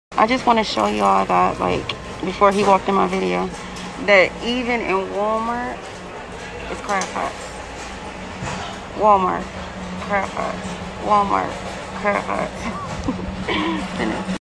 Crab pots in Walmart?!
Let me teach you how make $50-$60 an hour building Crab Pots